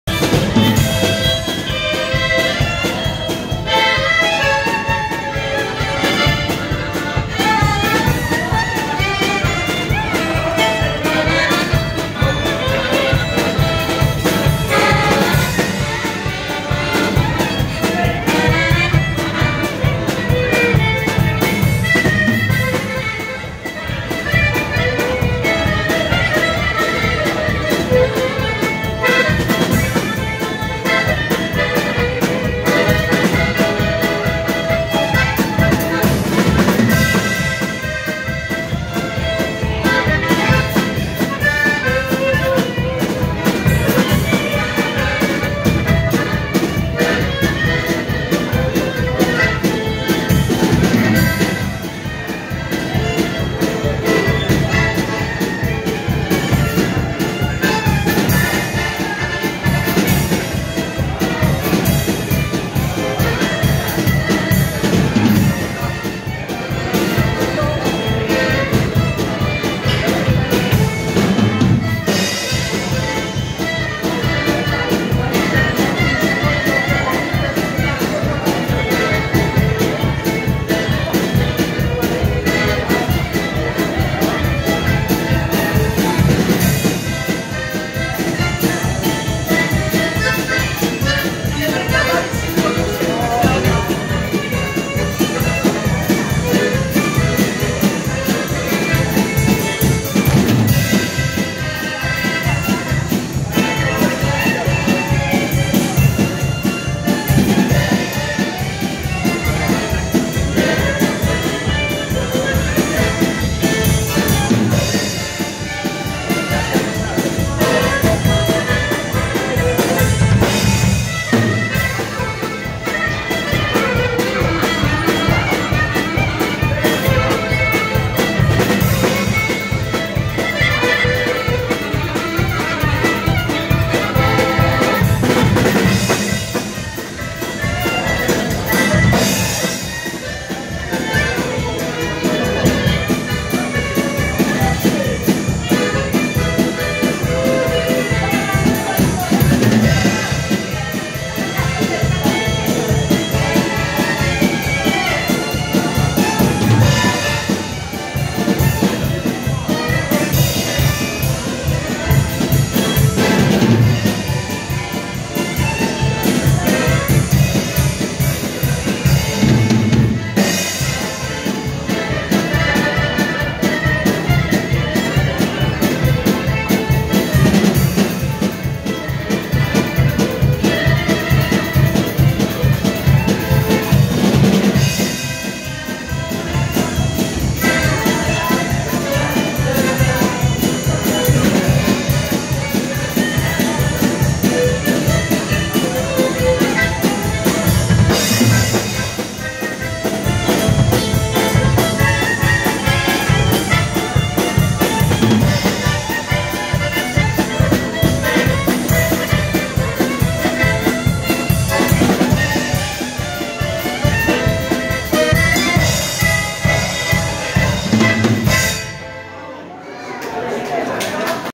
Kapela Zdzicha Kwapińskiego gra na potańcówce w Klubie Tanecznym "Ewa" w Radomiu.
Potańcówki odbywały się cyklicznie w karnawale 2020r i cieszyły dużą popularnością miłośników w muzyki ludowej.